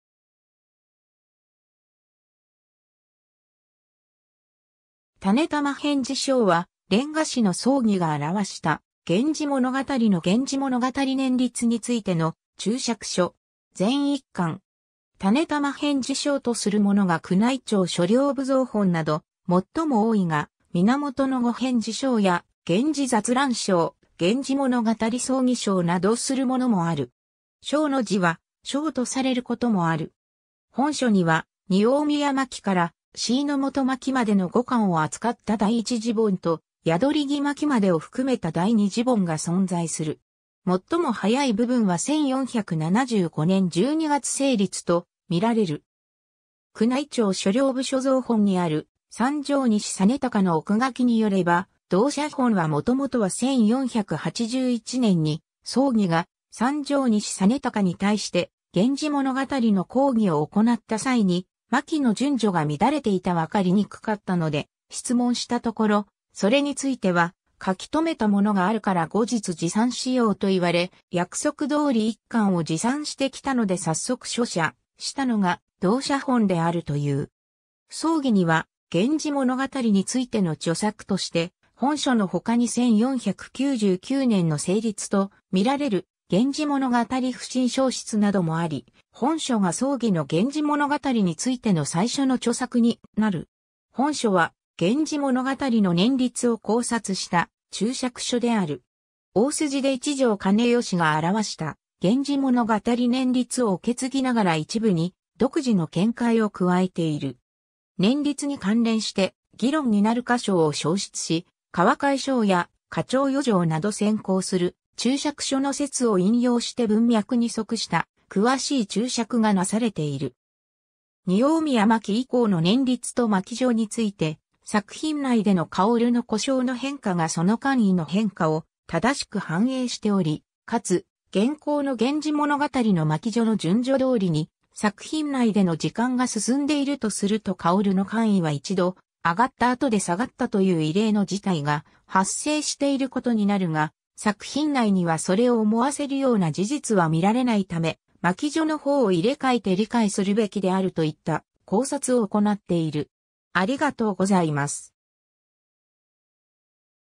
種玉編次抄
『種玉編次抄』（しゅぎょくへんじしょう）は、連歌師の宗祇が著した『源氏物語』の源氏物語年立についての注釈書。
全1巻。
 『種玉編次抄』とするものが宮内庁書陵部蔵本（桂宮本）など最も多いが、『源語編次抄』（げんごへんじしょう、桃園文庫蔵本）や『源氏雑乱抄』（げんじざつらんしょう、東北大学図書館蔵本）、『源氏物語宗祇抄』（げんじものがたりそうぎしょう、九州大学図書館蔵本）などするものもある。
「抄」の字は「鈔」とされることもある。
 本書には匂宮巻から椎本巻までの5巻を扱った第一次本と宿木巻までを含めた第二次本が存在する。
最も早い部分は1475年（文明7年）12月成立と見られる。
宮内庁書陵部所蔵本（桂宮本）にある三条西実隆の奥書によれば、同写本はもともとは1481年（文明13年）に宗祇が三条西実隆に対して源氏物語の講義を行った際に巻の順序が乱れていたわかりにくかったので質問したところ、「それについては書き留めたものがあるから後日持参しよう」と言われ、約束通り1巻を持参してきたので早速書写したのが同写本であるという。
宗祇には源氏物語についての著作として本書の他に1499年（明応8年）の成立とみられる『源氏物語不審抄出』などもあり、本書が宗祇の源氏物語についての最初の著作になる。
 本書は源氏物語の年立を考察した注釈書である。
大筋で一条兼良が著した「源氏物語年立」（いわゆる旧年立）を受け継ぎながら一部に独自の見解を加えている。
年立に関連して議論になる箇所を抄出し、『河海抄』や『花鳥余情』など先行する注釈書の説を引用して文脈に即した詳しい注釈がなされている。
 匂宮巻以降の年立と巻序について作品内での薫の呼称の変化がその官位の変化を正しく反映しており、かつ現行の源氏物語の巻序の順序通りに作品内での時間が進んでいるとすると薫の官位は一度上がったあとで下がったという異例の事態が発生していることになるが、作品内には（光源氏の須磨退去のような）それを思わせるような事実は見られないため、巻序のほうを入れ替えて理解するべきであるといった考察を行っている。